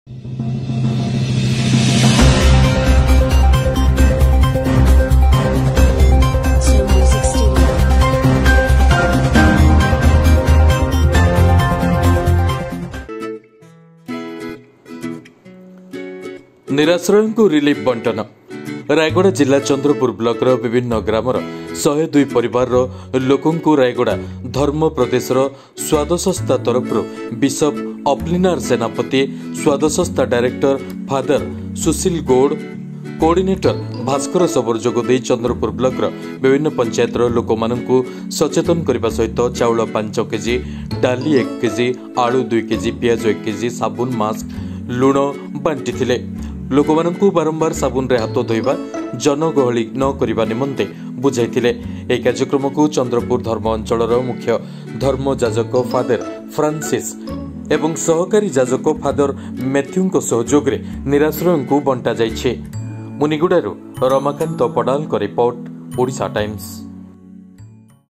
ରାୟଗଡା ଜିଲ୍ଲା ଚନ୍ଦ୍ରପୁର ବ୍ଲକ ରେ ନିରାଶ୍ରୟା ଙ୍କୁ ରିଲିଫ ବଣ୍ଟନ
ରାୟଗଡା ଜିଲ୍ଲା ଚନ୍ଦ୍ରପୁର ବ୍ଲକର ବିଭିନ୍ନ ଗ୍ରାମର ୧୦୨ ପରିବାର ରଲୋକମାନଙ୍କୁ  ରାୟଗଡା ଧର୍ମ ପ୍ରଦେଶର ସ୍ୱାଦସସ୍ଥା ତରଫରୁ  ବିଶପ ଆପ୍ଲିନାର ସେନାପତି ସ୍ୱାଦସସ୍ଥା ଡାରେକ୍ଟର ଫାଦେର ଶୁଶିଲ ଗୋଢ ,କଡିନଟର ଭାସ୍କର ଶବର ଯୋଗ ଦେଇ ଚନ୍ଦ୍ରପୁର ବ୍ଲକ ର ବିଭିନ୍ନ ପଞ୍ଚାୟତ ର ଲୋକ ମାନଙ୍କୁ ସଚେତନ କରିବା ସହିତଚାଉଳ,5kg ଡାଲି 1kg,ଆଳୁ2kg,ପିଆଜ 1kg,ସାବୁନ,ମାସ୍କ ,ଲୁଣଓ ଲୁଣ ବାଣ୍ଟିଦେଇଥିଲେ।ଲୋକ ମାନଙ୍କୁ ବାରମ୍ବାର ସାବୁନ ରେ ହାତ ଧୋଇବା ,ଜନଗହଳି ନକରିବା ନିମନ୍ତେ ବୁଝାଇ ଦେଇଥିଲେ।ଏହିକାର୍ଯ୍ୟକ୍ରମକୁ     ଚନ୍ଦ୍ରପୁର ଧର୍ମାଅଞ୍ଚଳର ମୂଖ୍ୟ ଧର୍ମଯାଜକ ଫାଦେର ଫ୍ରାନସିସ ଏ ସହକାରୀ ଯାଜକ ଫାଦେର ମାଥ୍ୟୁ ଙ୍କ ସହ ଯୋଗରେ  ନିରାଶ୍ରୟଙ୍କୁ ବାଣ୍ଟିଦିଆଯାଇଛି ।